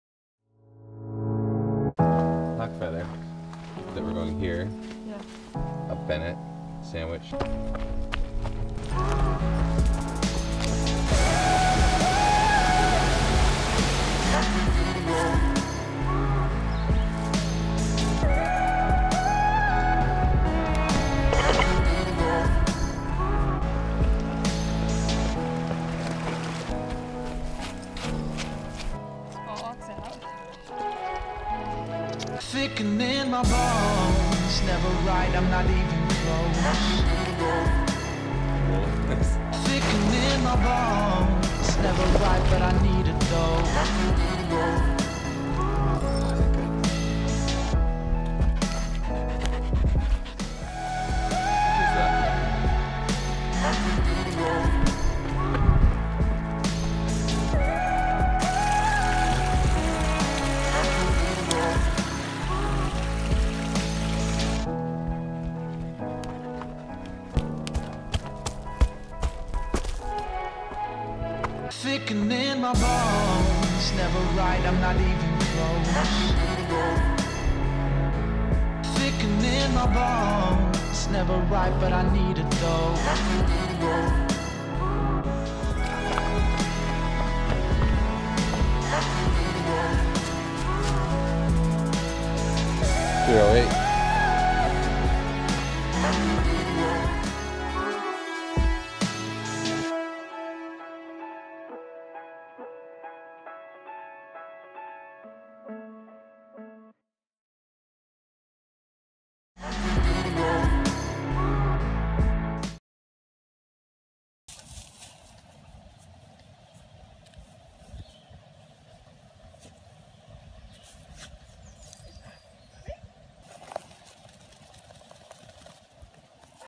Sandwich Mountain + Flat Mountain Pond
Pulled out a map of the Sandwich Range this weekend and looked for a mountain that I hadn't done yet. I'm almost certain I never hiked Sandwich Mountain (Sandwich Dome) since it's just shy of 4k' and is fairly isolated. We were looking for a trail run so we added on Flat Mountain Pond to check out the shelter. The old logging railroad provided the perfect grade for running with plenty of water for Squall along the way.

snap: sr_drummond